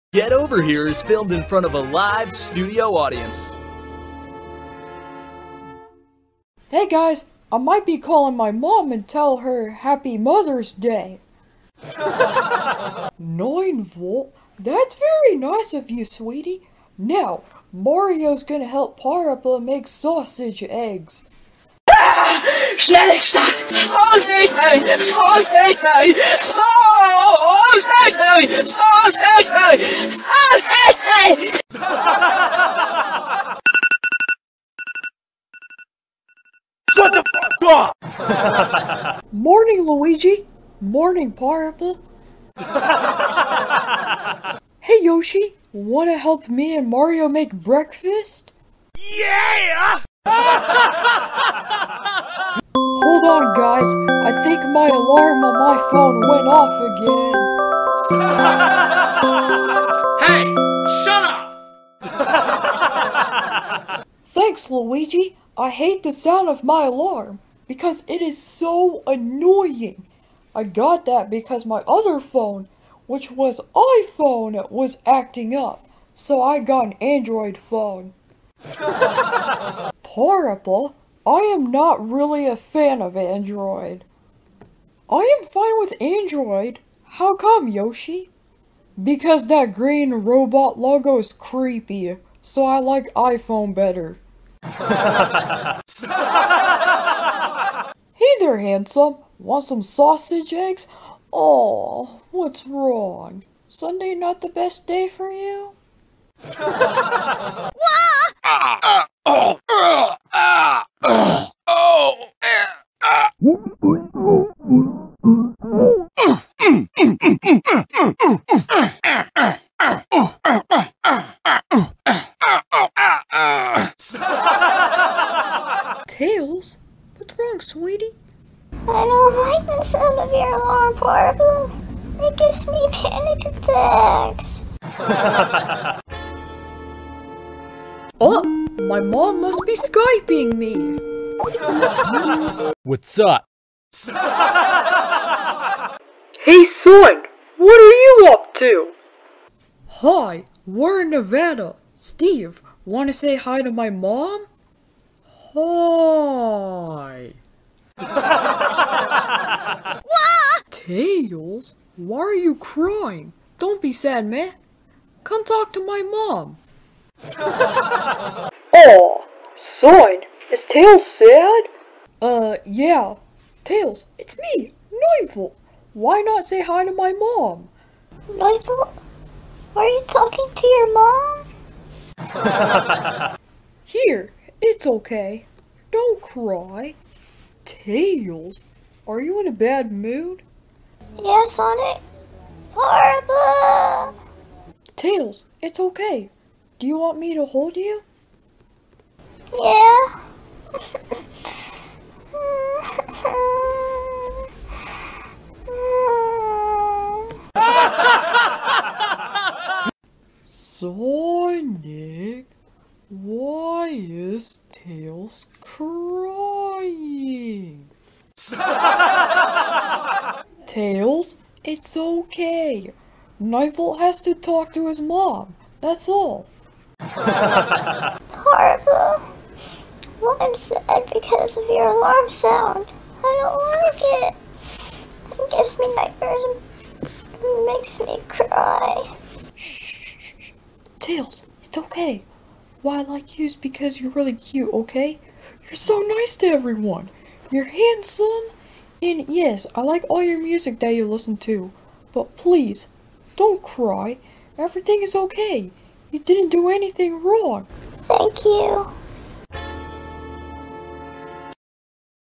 VGW Sitcom Movie: 9-Volt's Mother's Day
9-Volt skypes with his mom while Parappa and Mario make sausage eggs. Then, Parappa gets Tails and Tails cries. Then, 9-Volt, Steve, and Tails talk to his mom and Parappa holds Tails and comforts him.